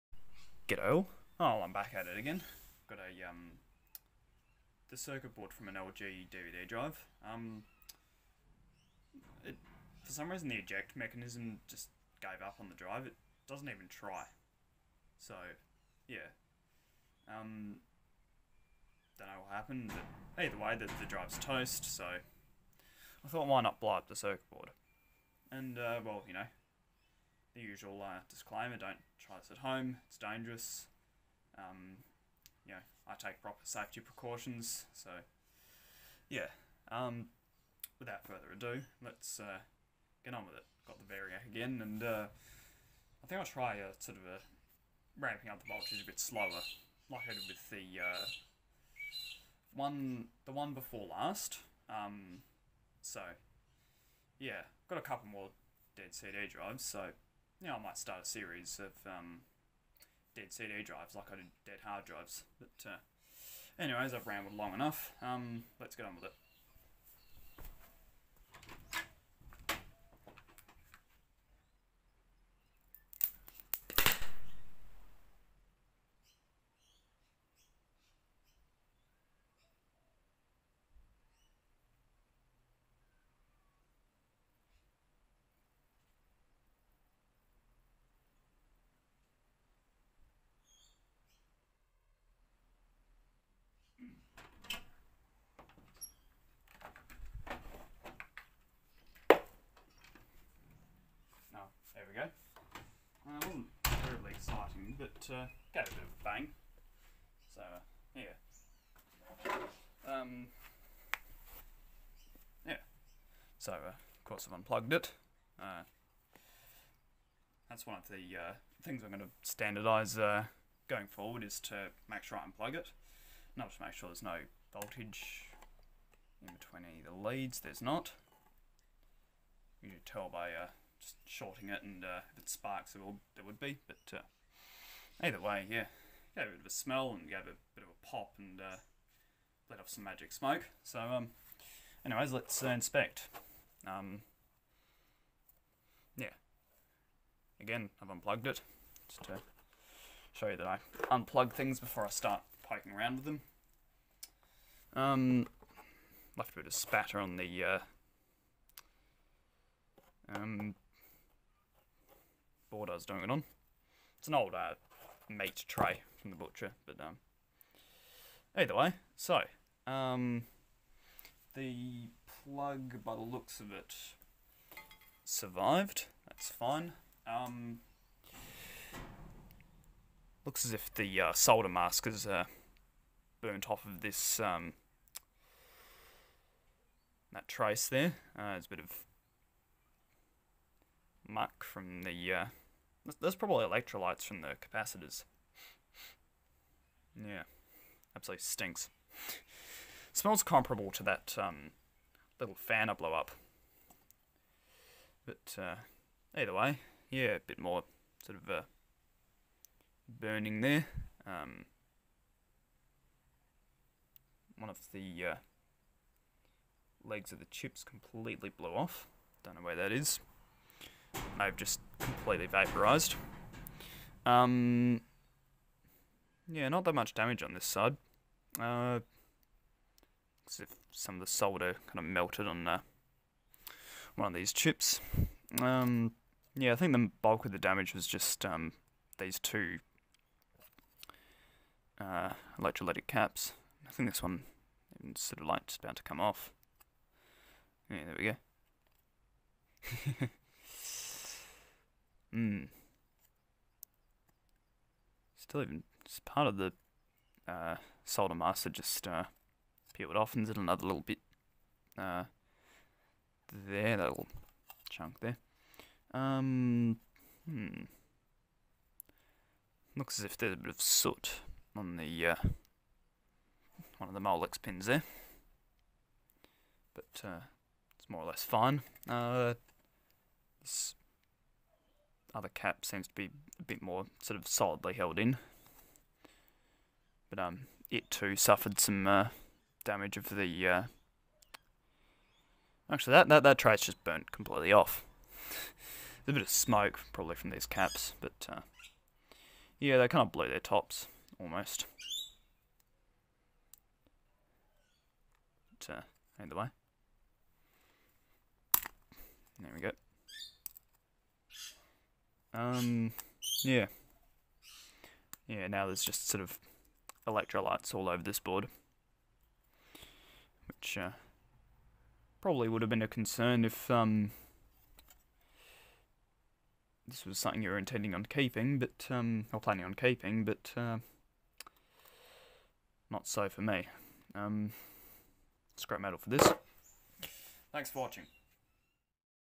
Dead DVD drive 1 - magic smoke and electrolytes
Not the most exciting one but not the dullest either. The electrolytes in the capacitors stunk of stale urine :P

At 1:58 when I said I was going to standardise unplugging it, what I meant to say was I was going to standardise showing I'd unplugged it. The former was standardised when I first started doing this.